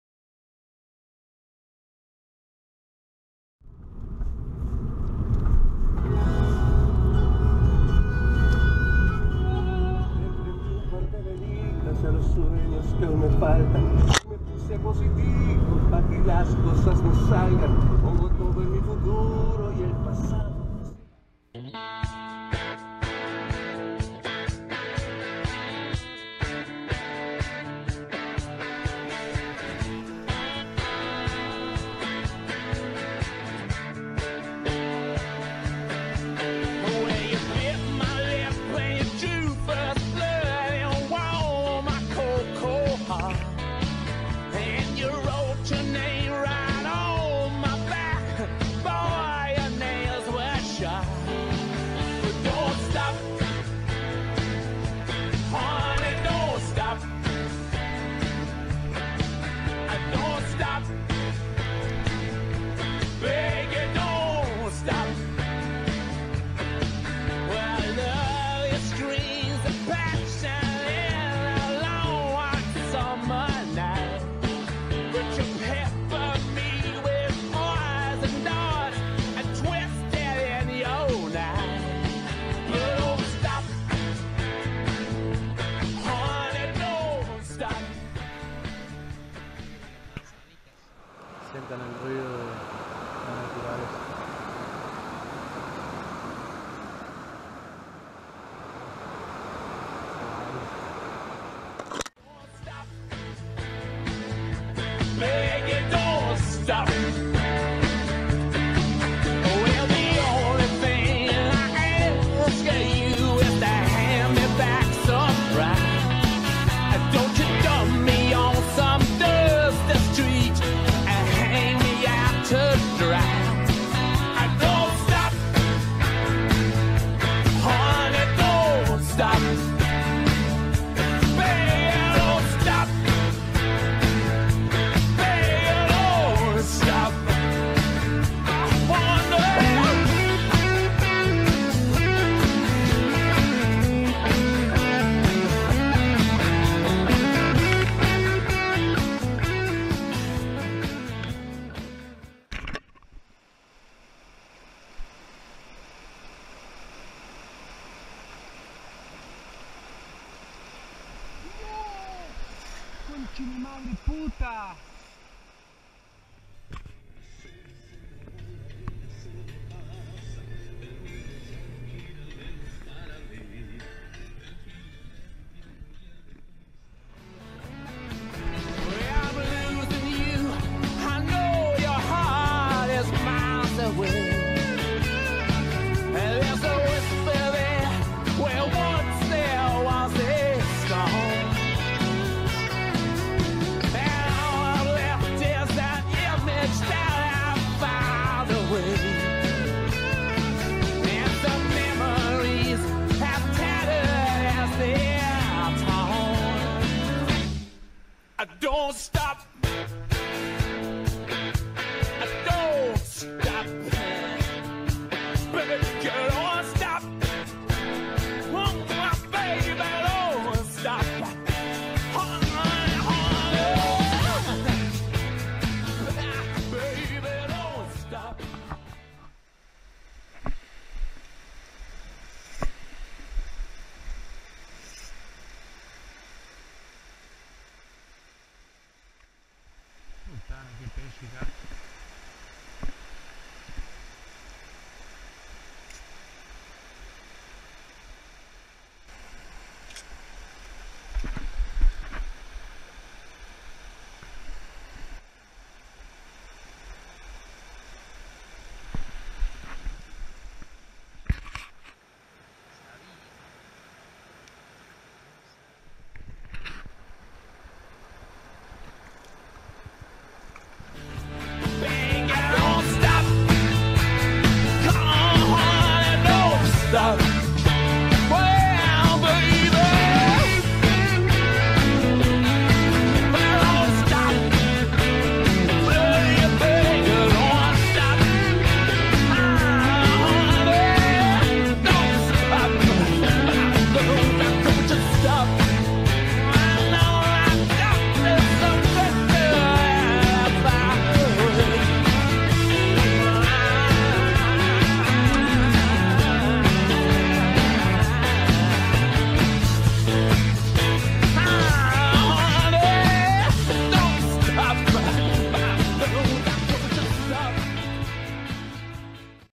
Trentino Fly Pesca 2023
Un par de dias de pesca con mosca recorriendo la zona del trentino, pescando los rios Noce y Avisio obteniendo algunas buenas capturas!

Un paio di giorni di pesca a mosca in Trentino, pescando nei fiumi Noce e Avisio e ottenendo alcune buone catture!

 A couple of days of fly fishing in the Trentino area, fishing the Noce and Avisio rivers and getting some good catches!

MiniFly 2023 Aperturas
Fly Fishing Italy